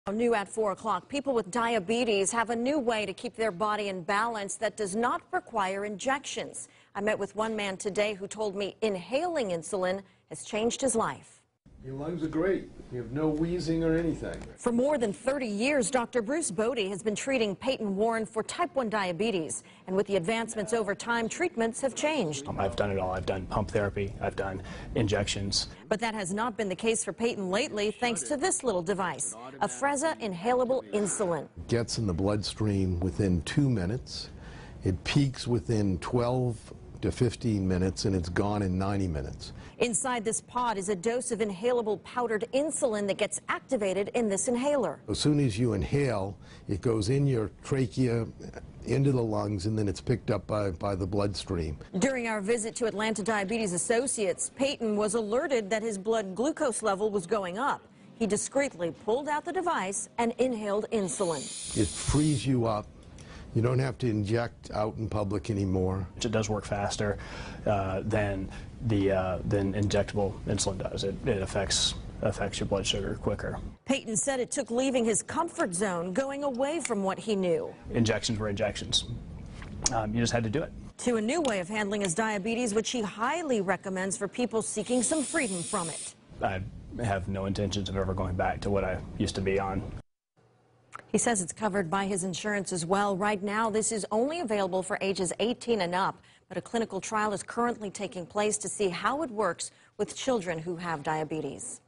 Diabetes patients have new way to take insulin
Afrezza allows one to inhale powdered insulin instead of using injectable insulin.